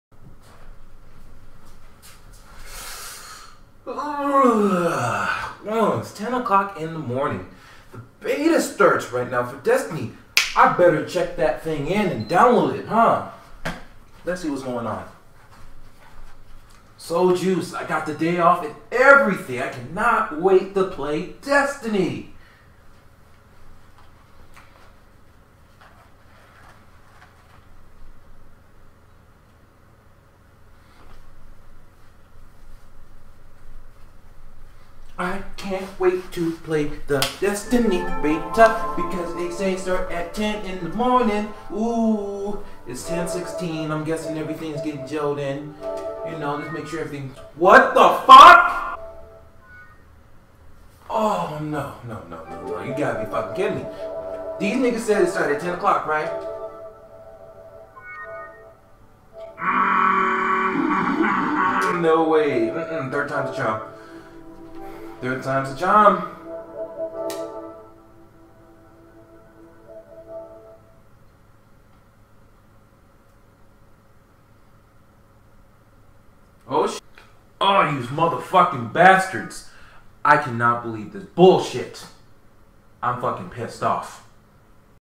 PSN is Down Can't Play Destiny Beta
PSN is down for maintenance. Stop lying to us and say the servers crashed and we are fixing it. Sorry you too your day off to not play the beta.

PSN is Down
PSN is Down
PSN is Down
PSN is Down
PSN is Down